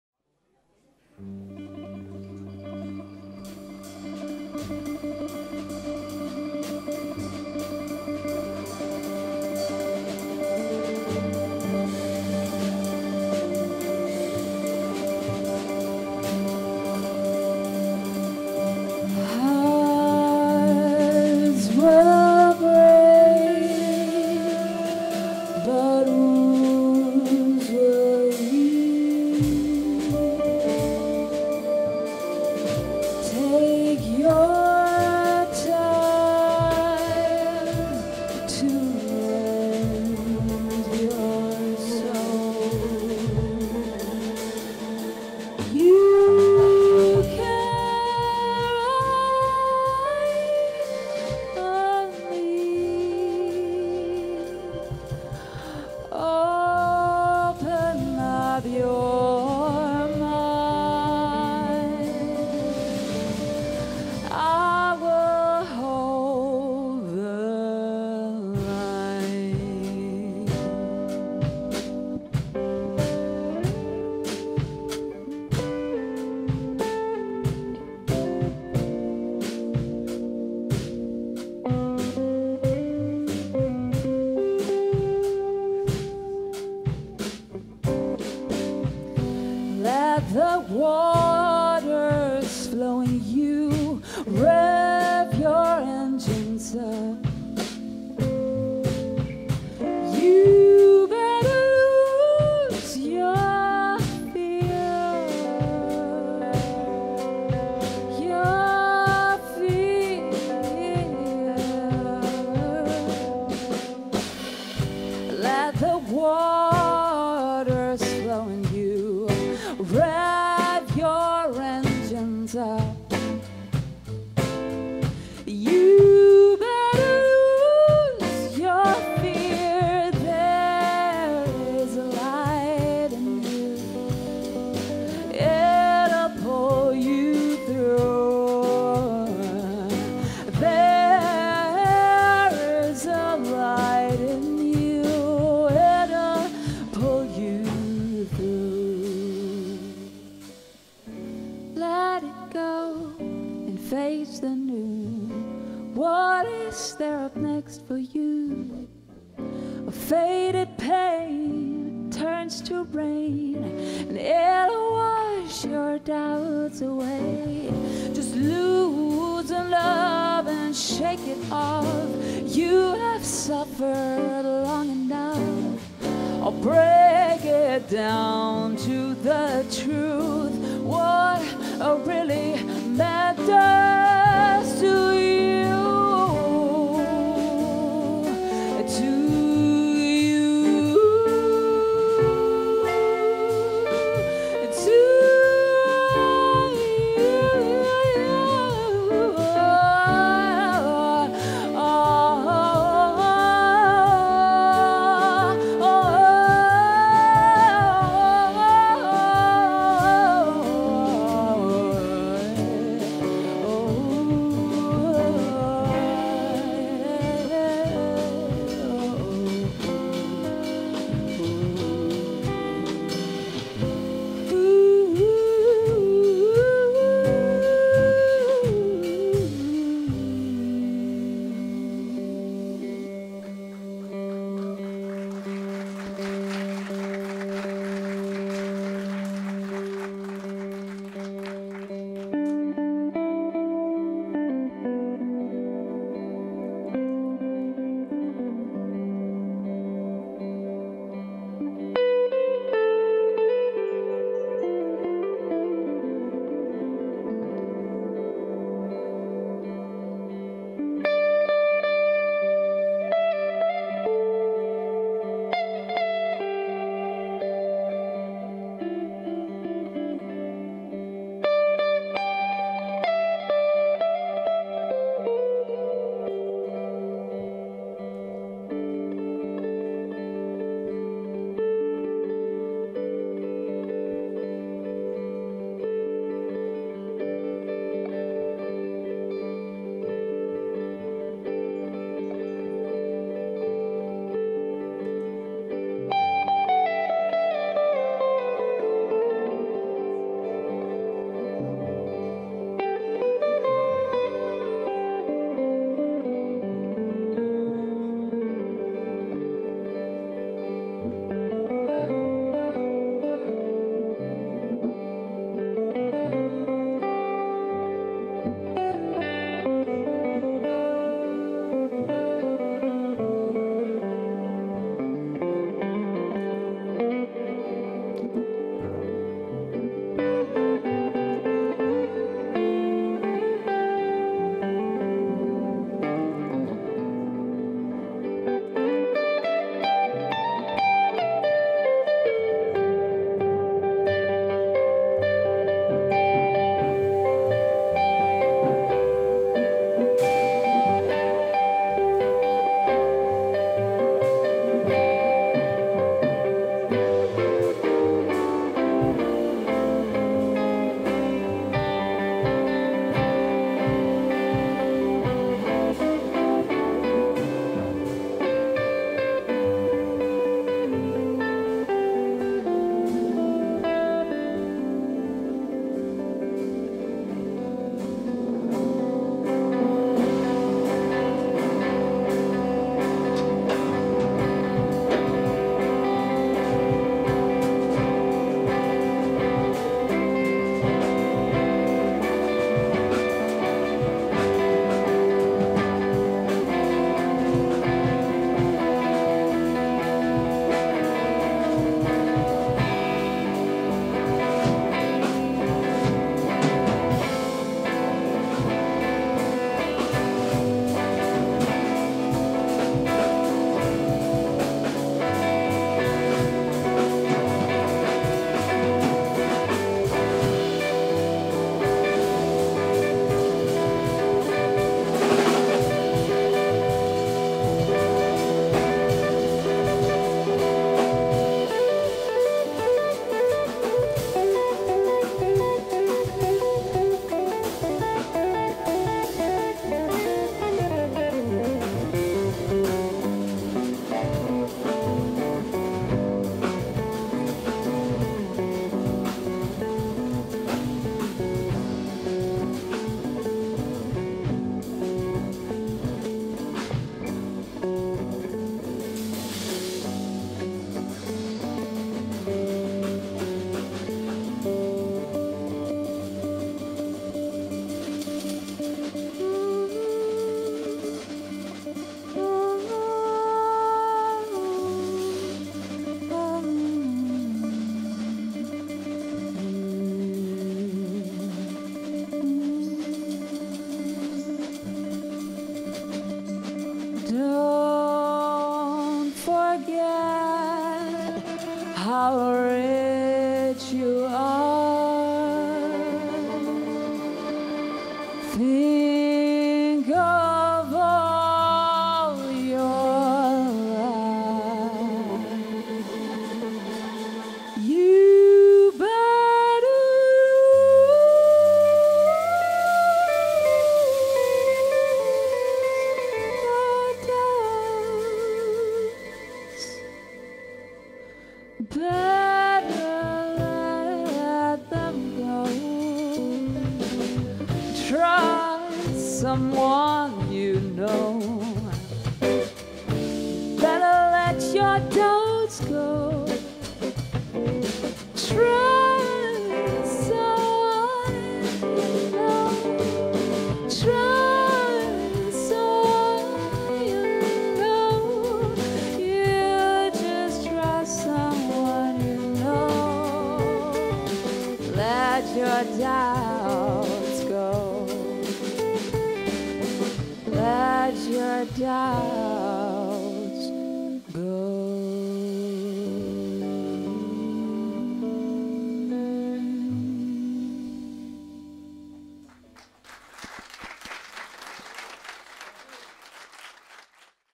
Marie Mokati au Casino Ajaltoun - Liban - Jeudi 17 Janvier
Pour l’ouverture de sa nouvelle saison 2019 placée sous le thème “places & memories” Le centre Culturel Ajaltoun CCA, présente un concert de Jazz "Marie Mokati"au Casino Ajaltoun

Marie Daniels - Vocal/Composition
Tim Bücher - Guitar
Moritz Götzen - Bass
Karl F. Degenhardt - Drums